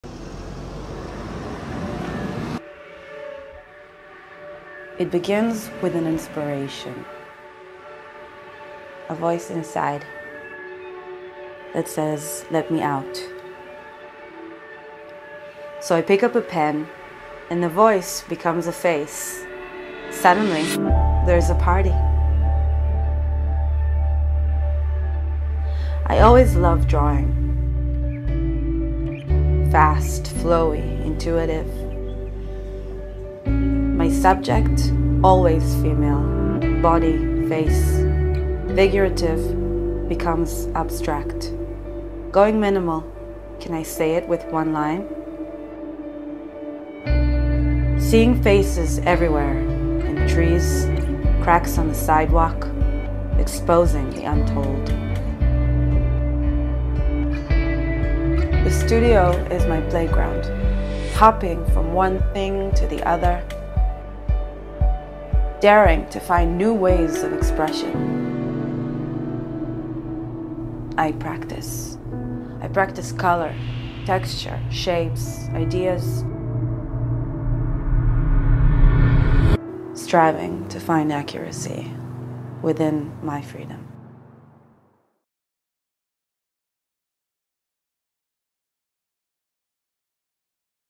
Artist Shira Barzilay & what inspires her
Tel Aviv Artist Shira Barzilay's (@koketit) one-line art is inspired by women as much as her environment. See how she brings her inspiration to life

Video by: Ori Taub (@taubori)